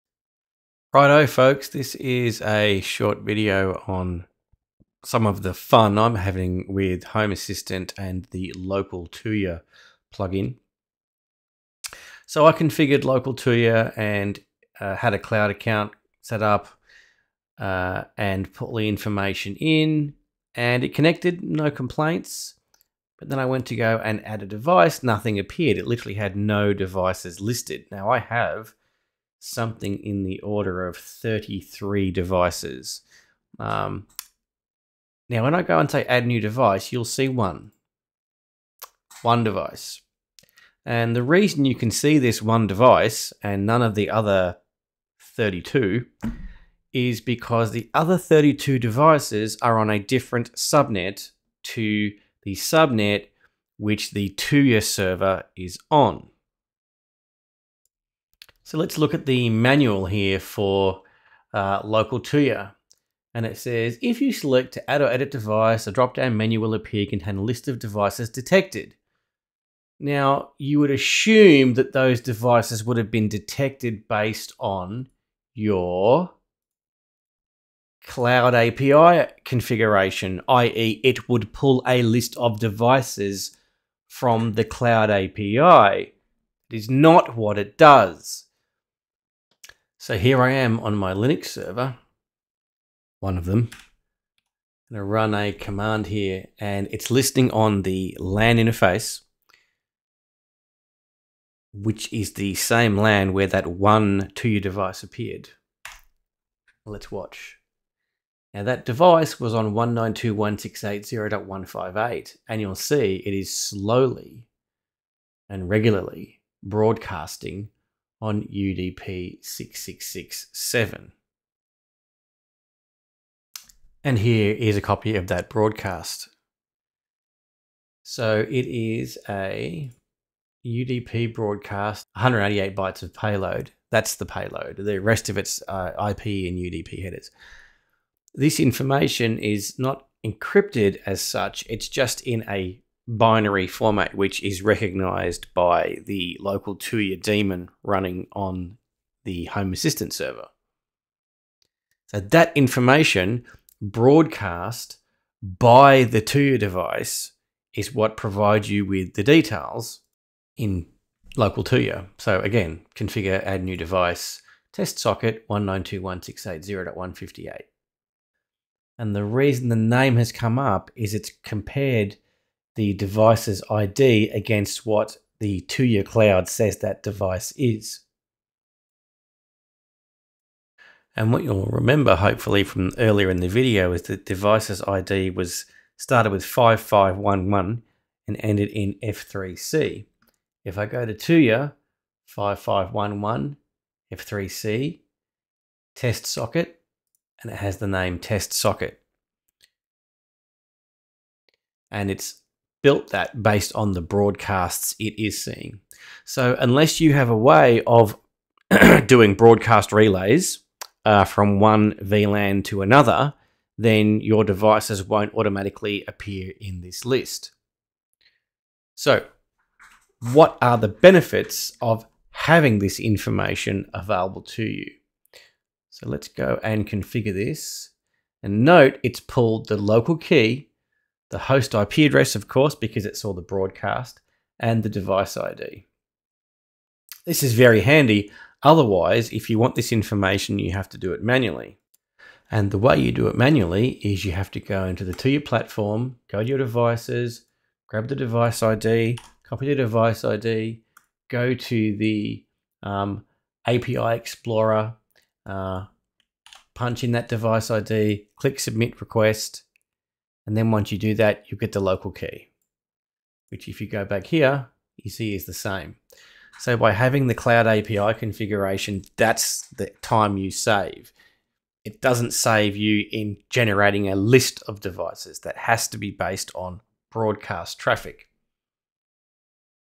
How LocalTuya discovers Tuya devices
The LocalTuya integration on Home Assistant can "discover" local Tuya devices.  However there is little to no documentation on how this process relates to whether you configure LocalTuya with your Cloud API details or not.

The summary is that the LocalTuya process listens for broadcasts on UDP port 6667.  If your Tuya devices are on another subnet (like mine all are, along with all of my other "IoT" devices), LocalTuya will not detect them.

This means you need to manually:
1. Find the device's IP address
2. Look up the device ID in the Tuya Cloud portal and copy it to the clipboard
3. Enter the API Explorer in the cloud portal, paste the Device ID and click Submit
4. Note down the Local Key
5. Manually add a new device in Local Tuya, typing in the IP address, Device ID, and Local Key

This is a bit of a pain in the ass.  I'm looking into a way to do broadcast forwarding using the network equipment that I have.